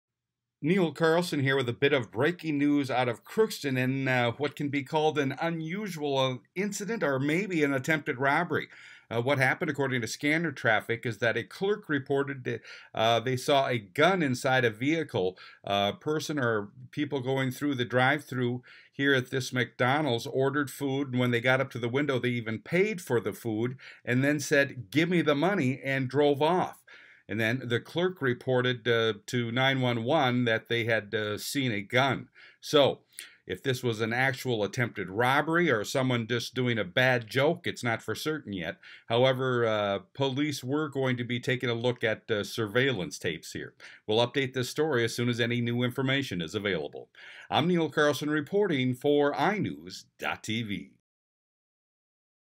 Attempted Robbery Or Joke?
CROOKSTON, MN (iNewZ.TV) Crookston Police are investigating a report of a gun a McDonald's drive through Monday evening.